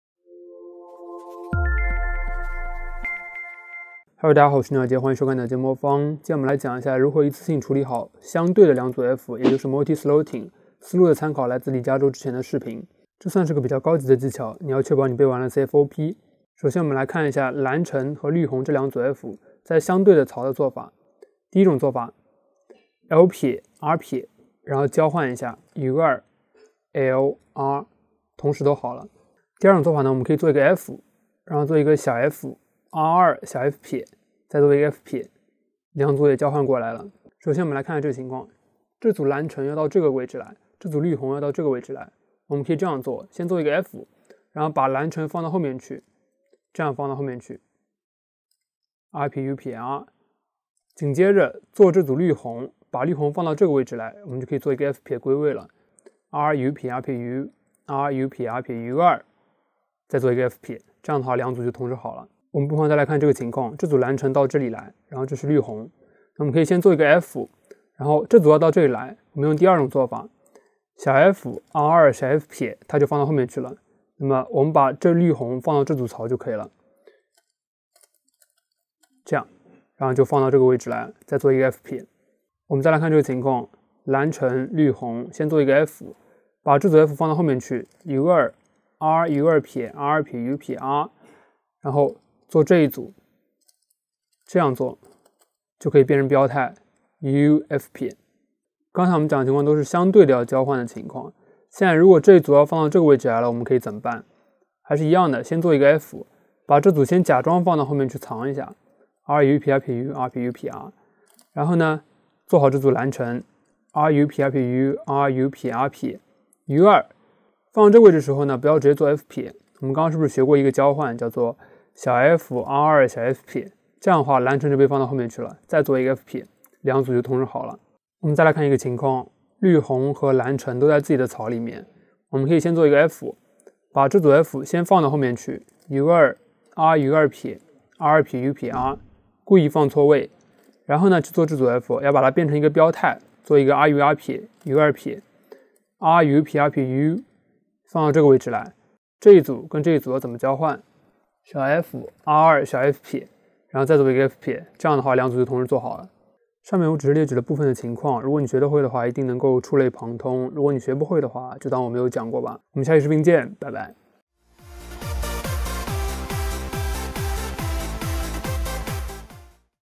【魔方进阶】如何同时处理相对槽的两组F？如何理解Multi-Slotting？
(L' R') U2 (L R) / F (f R2 f') F'

F U' (R U' R' U R' U' R)
U2 (R U' R' U) (R U' R' U2) F'

F (f R2 f')
U' F (U R U' R') F' F'

F U2 (R U2' R2' U' R) U2
R U R' U2' R U' R' U F'

F U' (R U' R' U R' U' R) U2
(R U' R' U) (R U' R' U2) (f R2 f') F'

R U R' U2' R U' R' U (f R2 f') F'